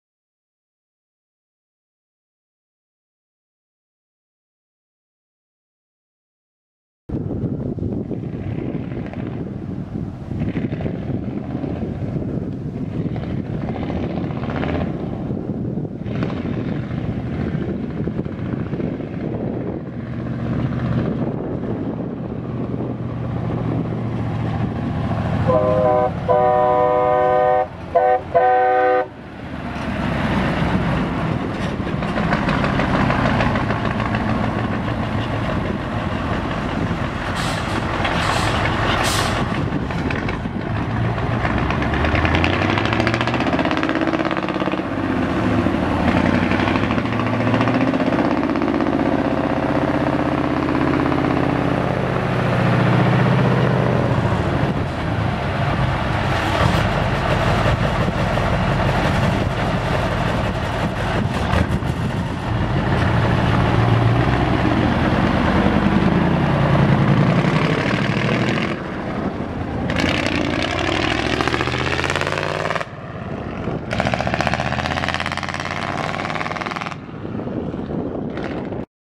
1971 Autocar A64B semi truck for sale | sold at auction July 22, 2014
1971 Autocar A64B semi truck
104,947 miles on odometer
Cummins 903 V8 diesel engine
13 speed manual transmission
Twin screw
Heat
Air suspension seat
Train horn
Utility lights
Butterfly grill
Wood floor
Visor
Full fiberglass fenders
Spring suspension
Air brakes
Wheel and nut wheel covers
Aluminum wheels
Goodyear 315/80R22 front tires
Bridgestone M880 11R22.5 rear tires
Truck has been restored
AB006HB068743